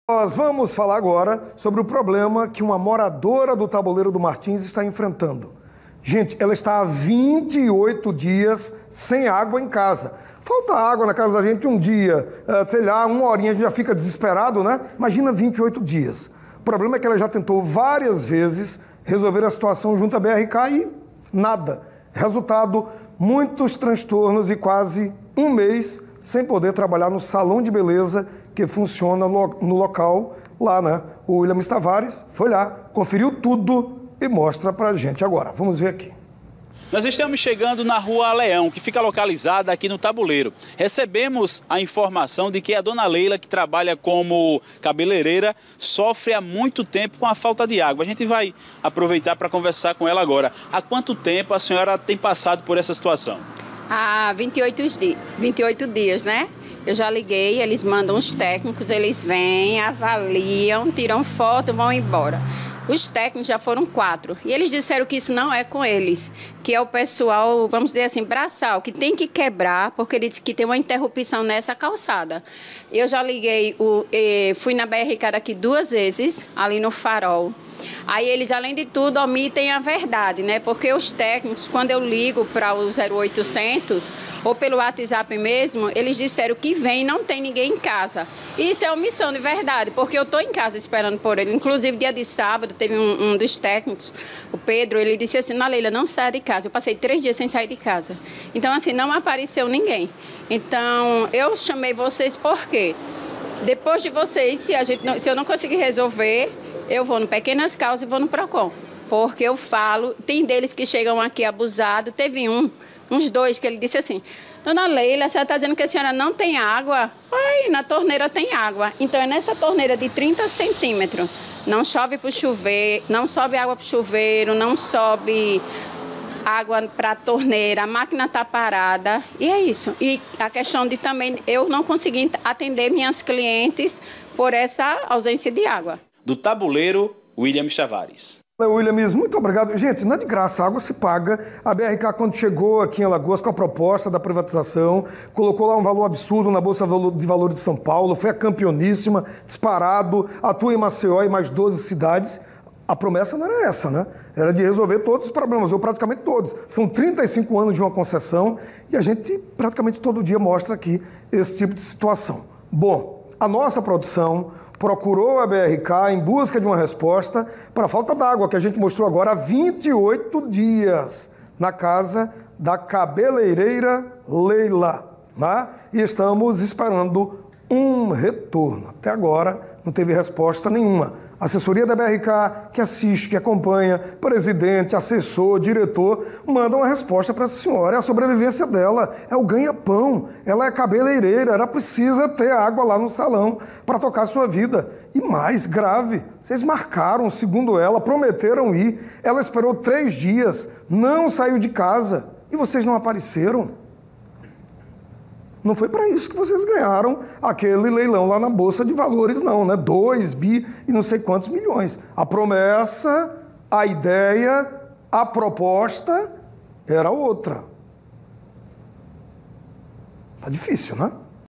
Moradora do Tabuleiro do Martins reclamam que está sem água há quase um mês
Veja mais detalhes sobre a falta de água, na reportagem de Williames Tavares.